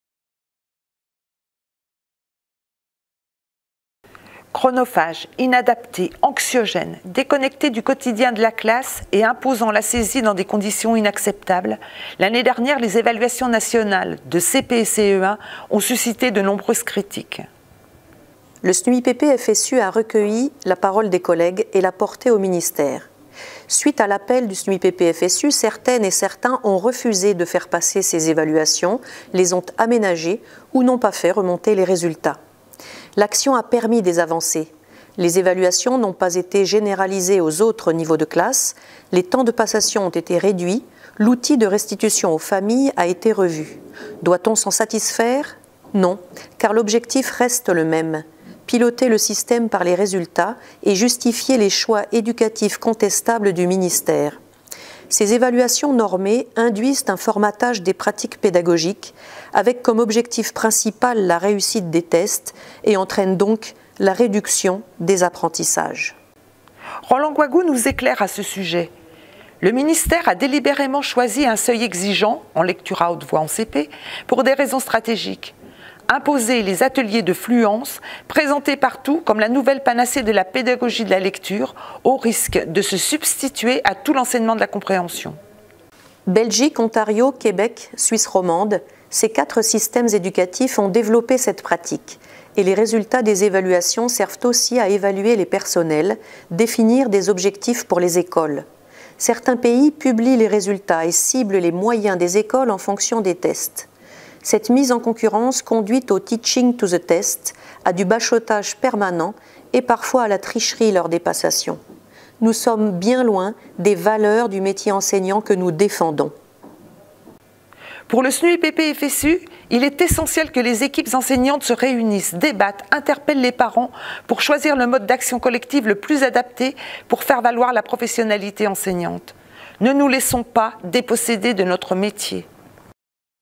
Evaluations CP-CE1 2019 : un pilotage par les résultats
Le ministère impose de nouveau des évaluations nationales aux CP et CE1 à partir du 16 septembre. Face aux critiques, il y a eu quelques avancées mais derrière ce toilettage l'objectif reste le même.